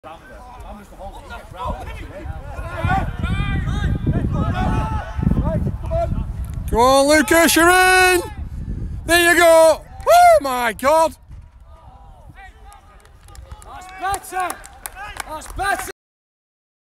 Astley & Buckshaw v CarnForth Rangers - Chance 4
Astley & Buckshaw Utd play in the West Lancashire Division 1 in the North West of England.